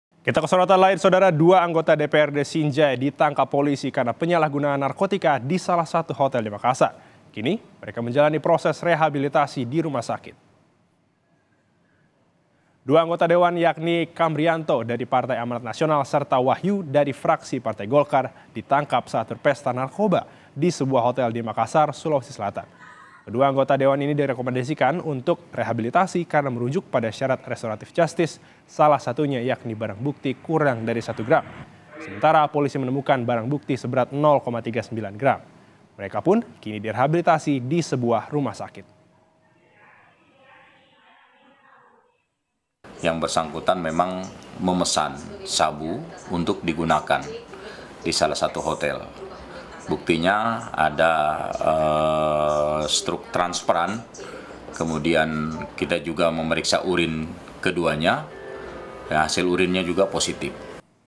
Dua Anggota DPRD Sinjai Ditangkap saat Pesta Narkoba di Hotel, Pelaku Direhabilitasi
MAKASSAR, KOMPAS.TV - Dua anggota DPRD Sinjai ditangkap polisi karena penyalahgunaan narkotika di salah satu hotel di Makassar. Kini mereka menjalani proses rehabilitasi di rumah sakit.

Dua anggota dewan yakni Kamrianto dari Partai Amanat Nasional serta Wahyu dari fraksi Partai Golkar ditangkap saat berpesta narkoba di sebuah hotel di Makassar, Sulawesi Selatan.

Kedua anggota dewan ini direkomendasikan untuk di rehabilitasi karena merujuk pada syarat restorative justice salah satunya yakni barang bukti kurang dari 1 gram.

Sementara polisi menemukan barang bukti seberat 0,39 gram. Mereka pun kini direhabilitasi di sebuah rumah sakit.